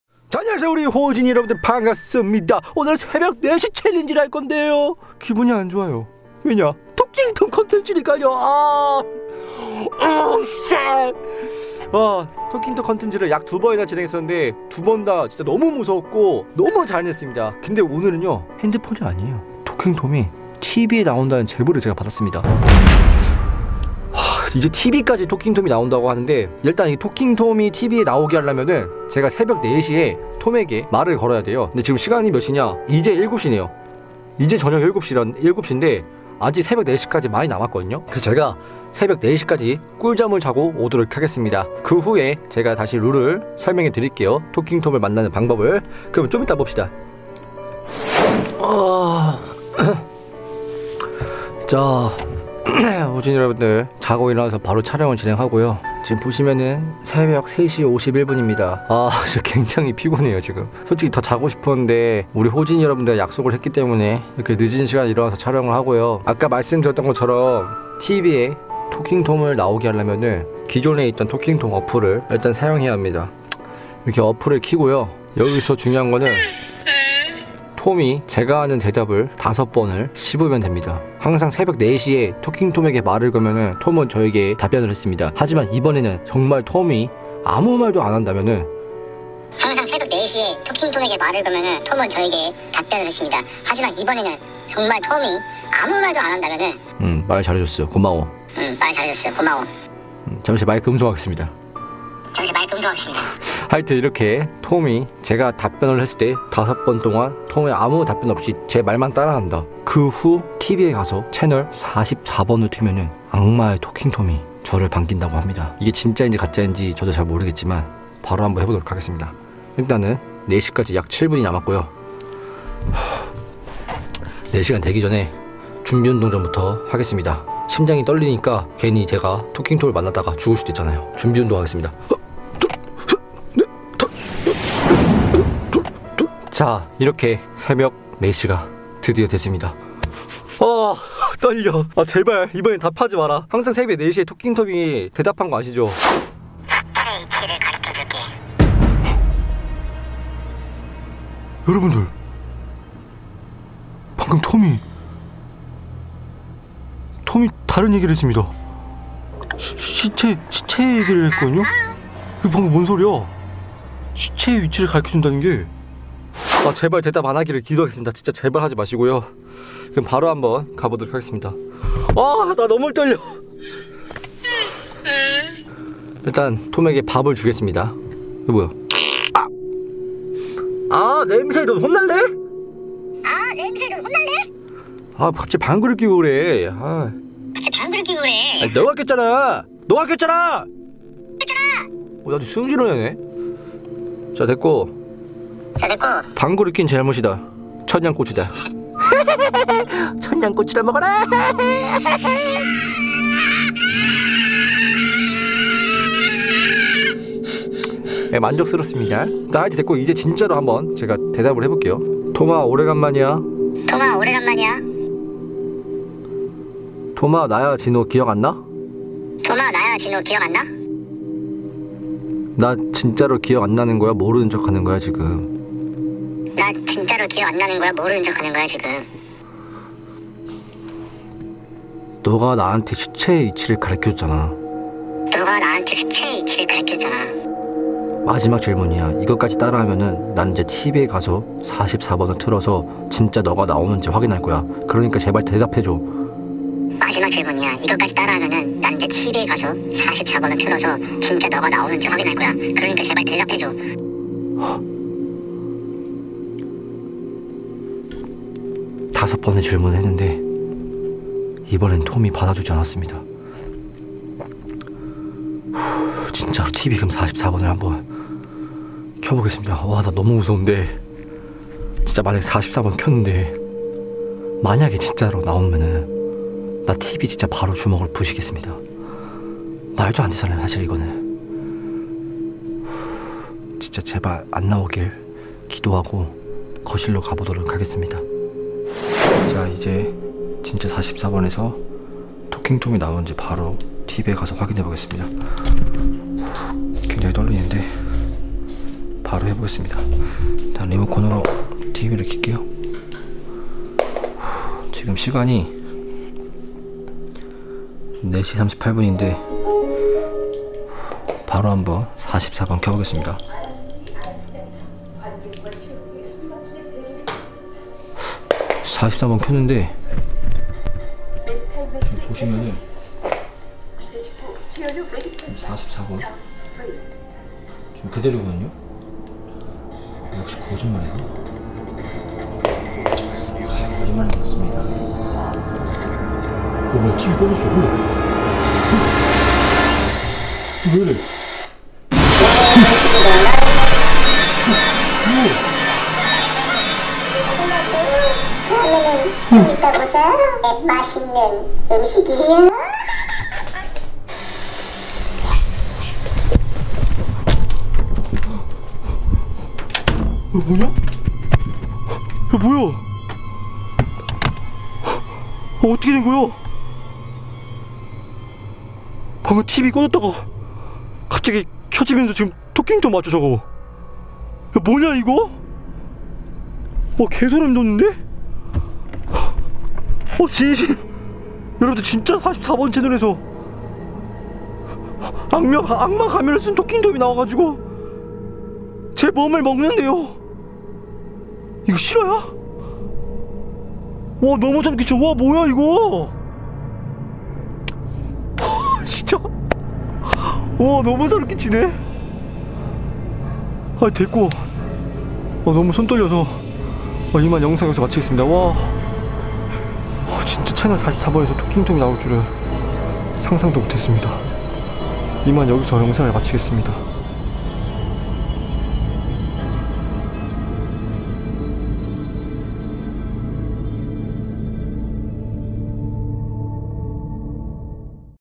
TV에서 토킹 톰이 등장 했습니다 내 심장을 먹는다고..?!! -진호-
이 영상은 상황극 입니다.